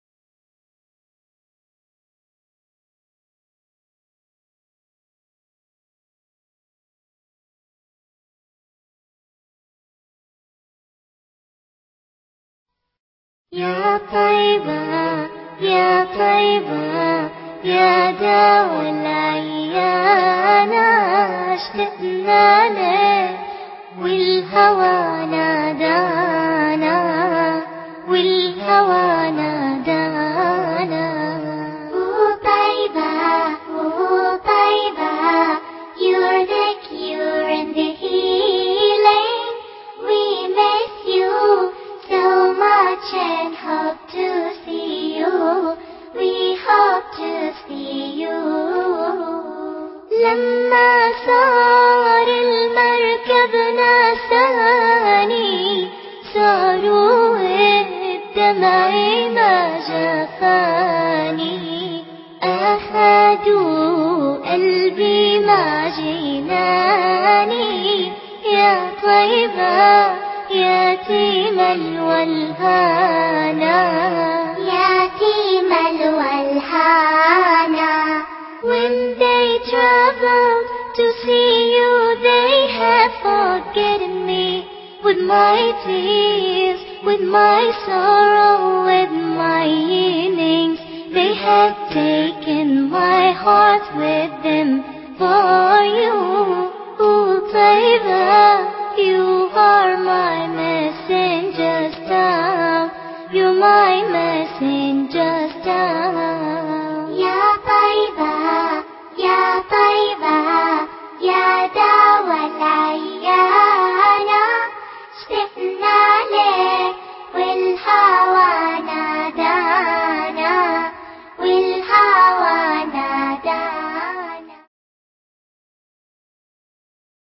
Lyon fête l'Aïd 2010 (1ère édition)
Lyon fête l'Aïd au Double Mixte de Villeurbanne - Campus de la Doua.
La fête de l'Aïd 2010 fut un succès en accueillant près de 5000 visiteurs, alors on compte sur vous en 2011 au Double Mixte!!